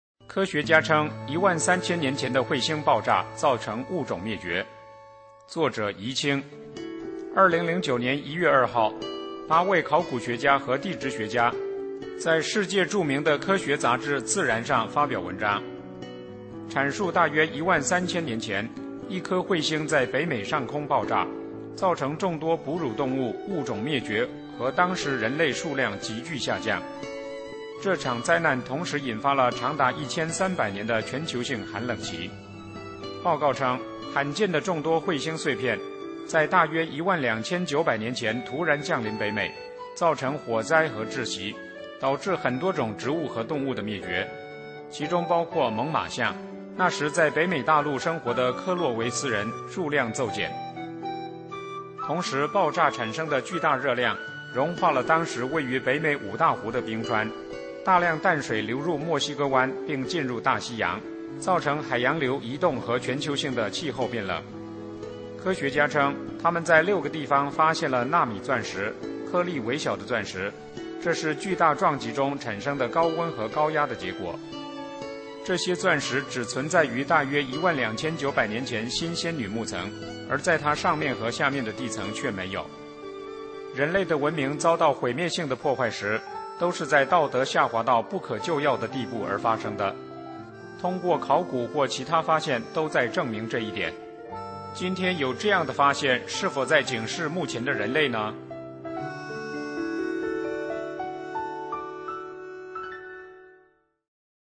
科學家稱一萬三千年前的彗星爆炸造成物種滅絕
作者：儀清
【正見網2009年01月04日】2009年1月2日，八位考古學家和地質學家在世界著名的科學雜誌《自然》上發表文章，闡述大約一萬三千年前，一顆彗星在北美上空爆炸，造成眾多哺乳動物物種滅絕和當時人類數量急劇下降。這場災難同時引發了長達一千三百年的全球性寒冷期。

在北美土壤中發現鑽石表明彗星導致的幾十種哺乳動物被滅絕

報告稱，「罕見的眾多彗星碎片」在大約一萬兩千九百年前突然降臨北美，造成火災和窒息，導致很多種植物和動物的滅絕，其中包括猛□象。那時在北美大陸生活的科洛維斯人（Clovis)數量驟減。
同時，爆炸產生的巨大熱量融化了當時位於北美五大湖的冰川。大量淡水流入墨西哥灣並進入大西洋，造成海洋流移動和全球性的氣候變冷。
科學家稱他們在六個地方發現了納米鑽石（顆粒微小的鑽石），這是巨大撞擊中產生的高溫和高壓的結果。這些鑽石只存在於大約一萬兩千九百年前新仙女木層 （Younger Dryas Boundary layers)，而在它上面和下面的地層卻沒有。
人類的文明遭到毀滅性的破壞時，都是在道德下滑到不可救要的地步而發生的。通過考古或其它發現都在證明這一點，今天有這樣的發現是否在警示目前的人類呢？